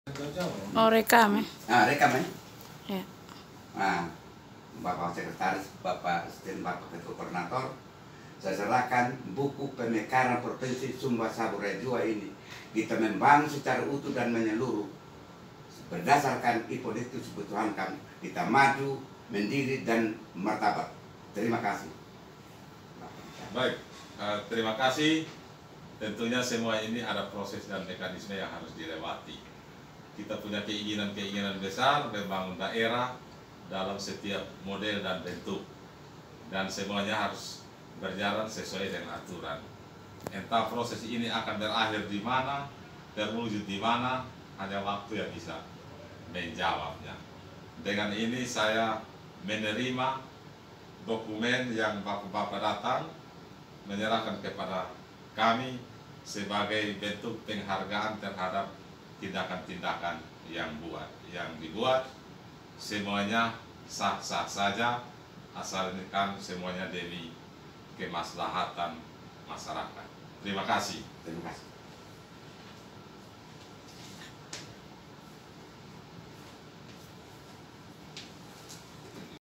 Sekretaris Inisiator Pemekaran Propinsi Sumba-Sabu Raijua Menyerahkan Berkas atau Dokumen
Sekretaris Inisiator DOB Propinsi Sumba-Sabu Raijua, Menyerahkan Dokumen Pemekaran (11/07/2023)